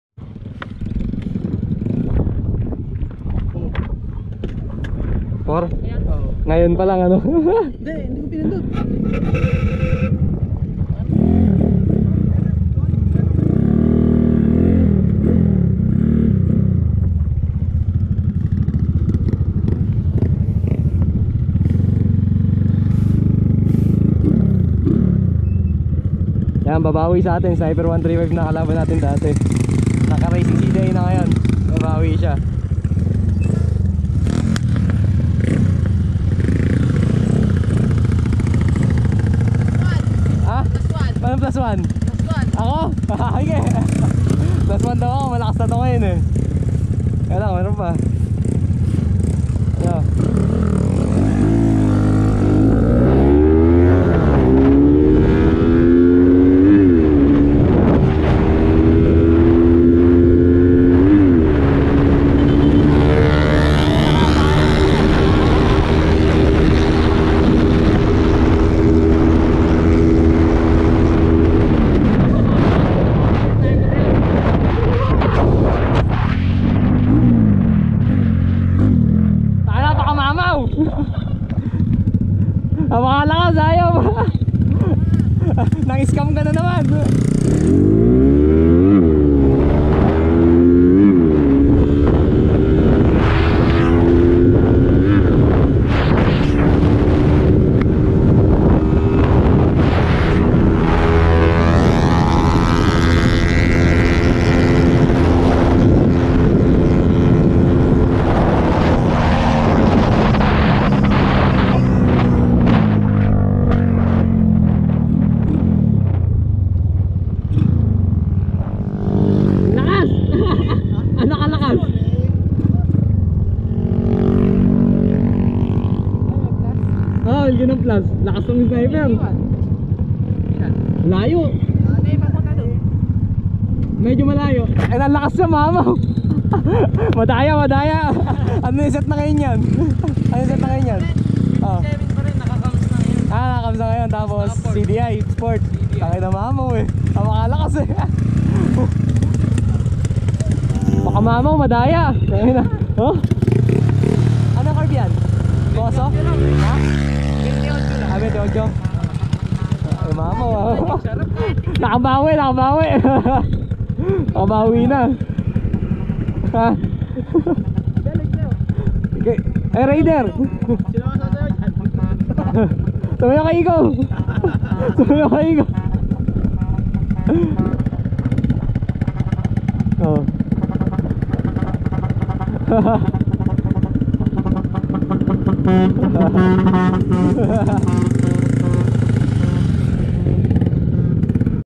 SNIPER 135 CLASSIC VS SMASH 115 | BUMAWI! NAPOSTEHAN AKO!
sniper 135 classic bumawi! binigyan na ng plus iniwan pa ng dalawang poste!